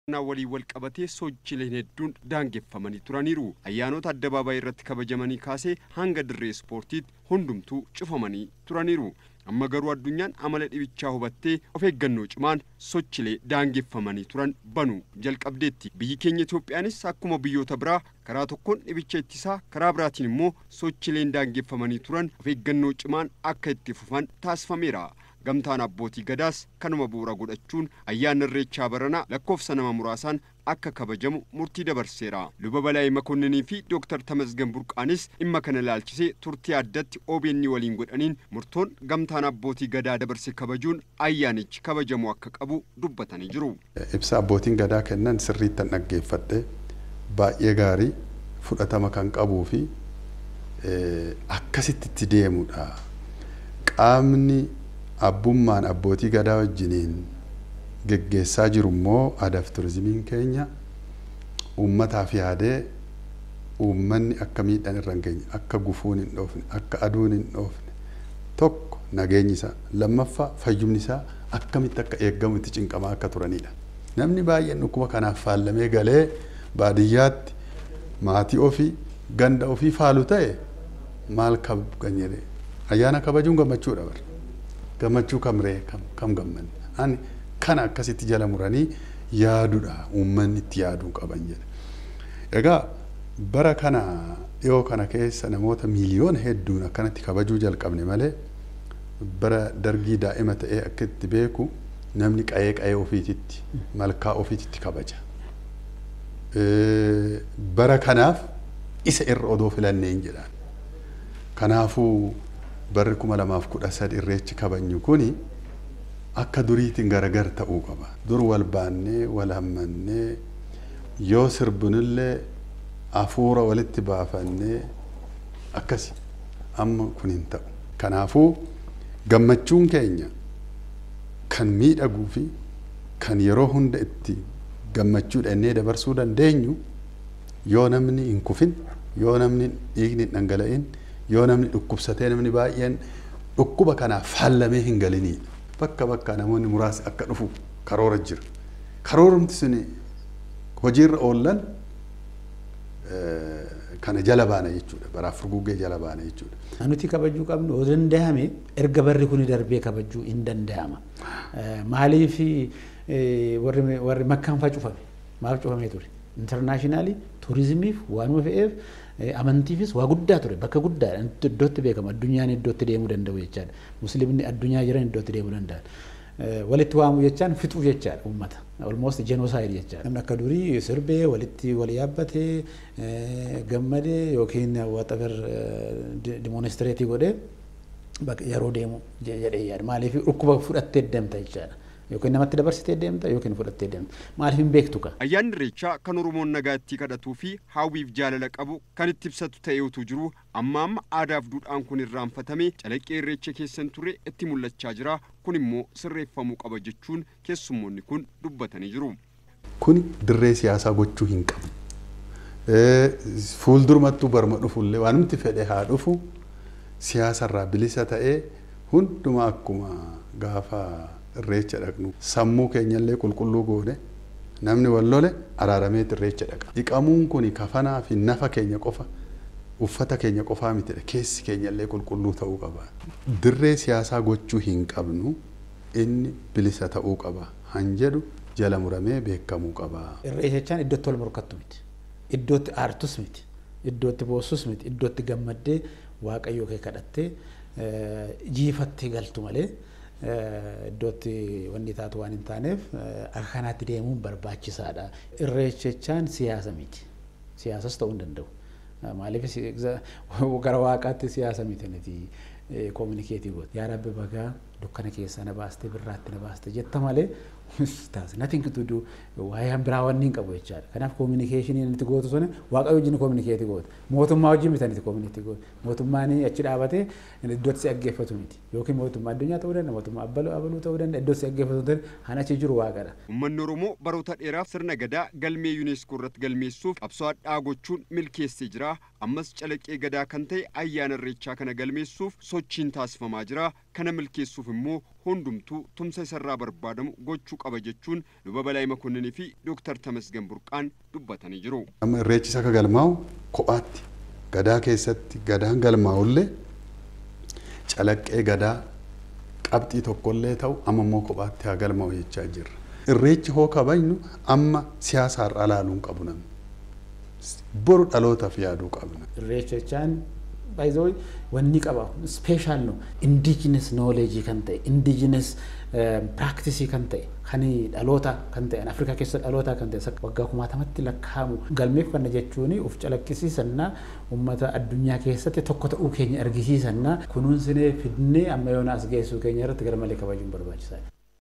Ayyaana Irreechaa.
Ayyaanni  Irreechaa kallattii gamtaa Abbootiin  Gadaa kaa’aniin kabajamuu akka qabuu fi galma ga’iinsa    UNESCO’tti galmeessisuuf qabame milkeessuuf    uummanni   tumsuu qaba jedhan, kessuummoonni  Istuudiyoo Finfineetti mari’atan.